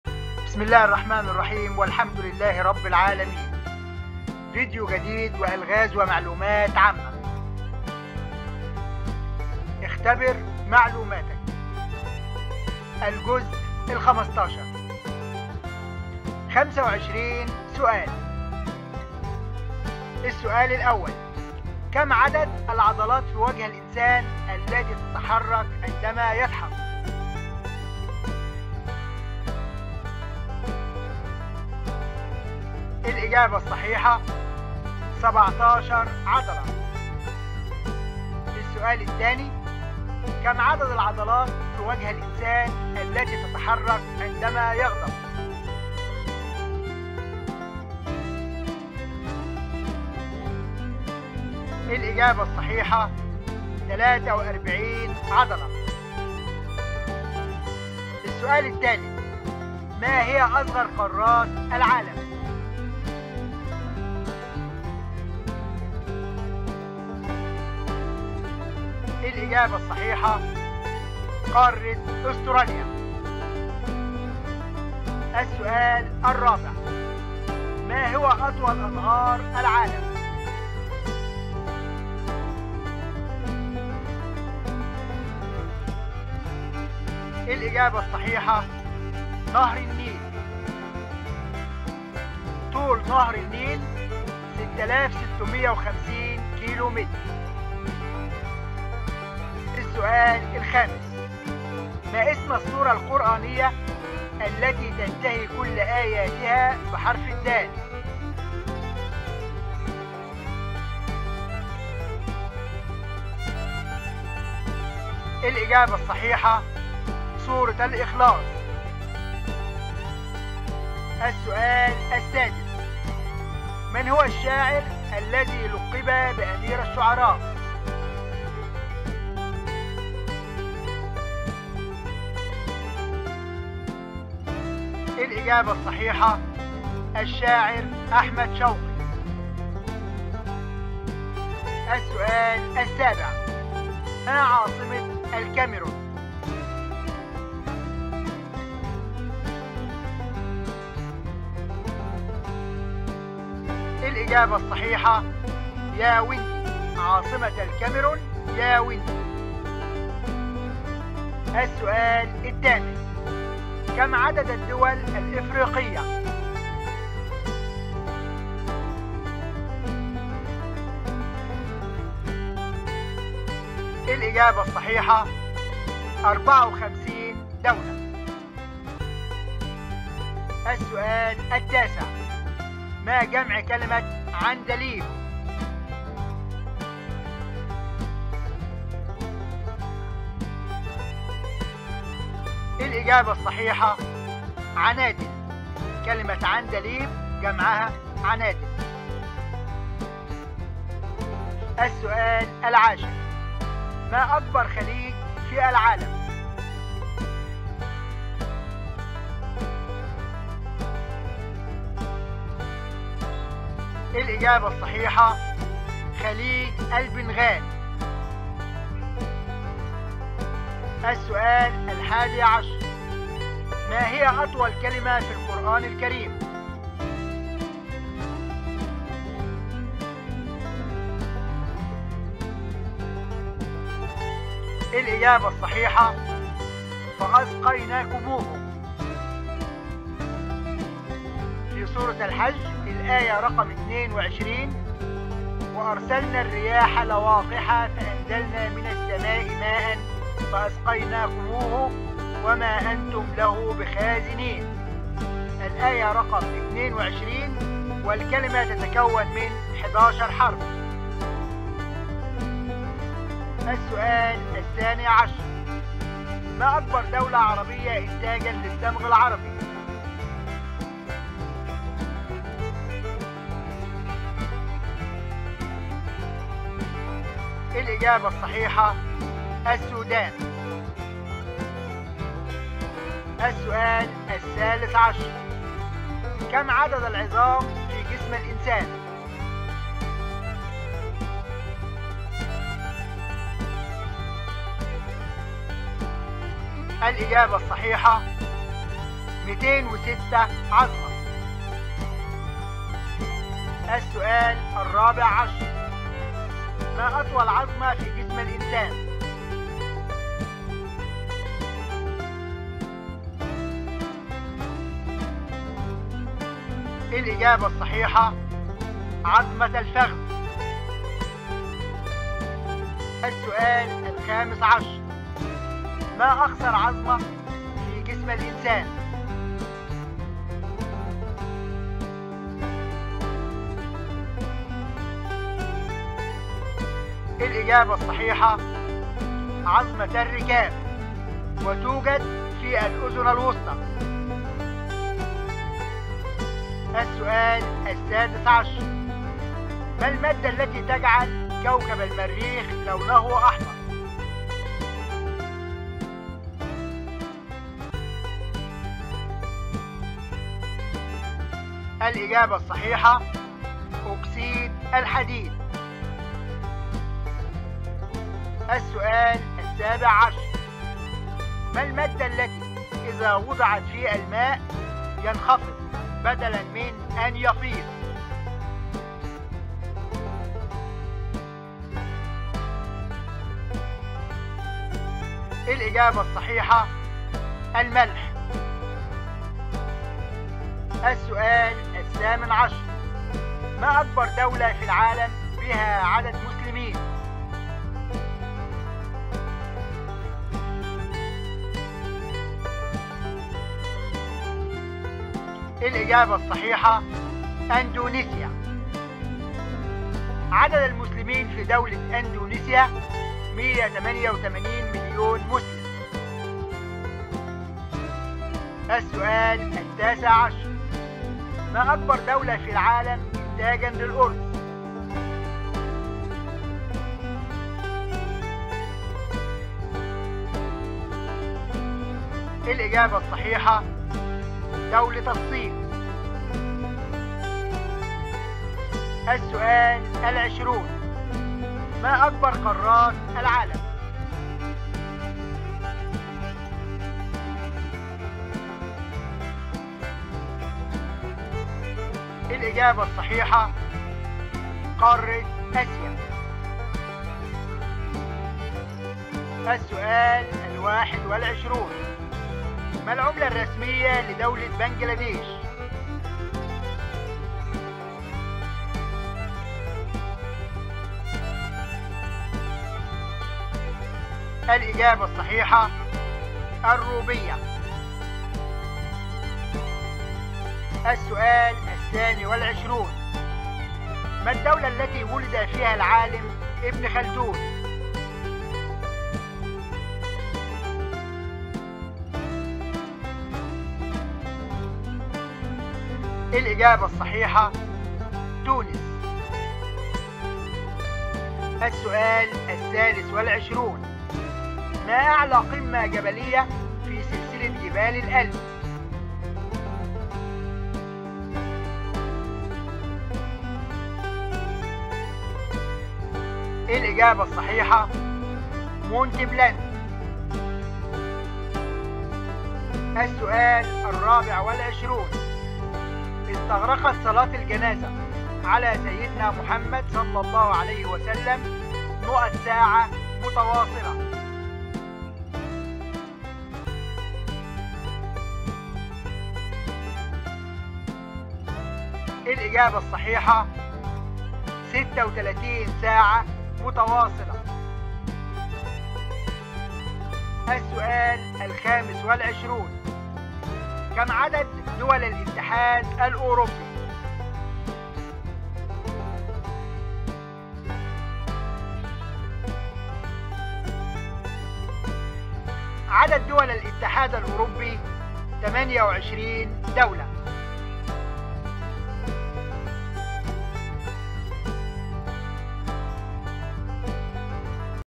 ألغاز ومعلومات ( الجزء 15 )
25 سؤال معلومات عامة - رياضية - ثقافية - جغرافية - علمية - دينية